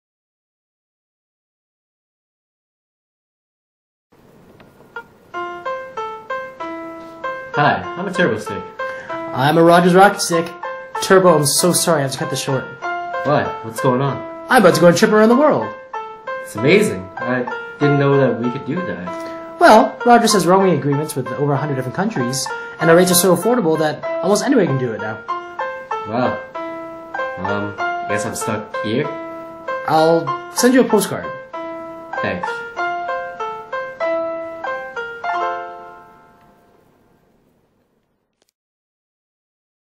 Geek Speak Digital Short: Rocket Stick (Roaming)
Turbo and Rocket talk about Global Roaming.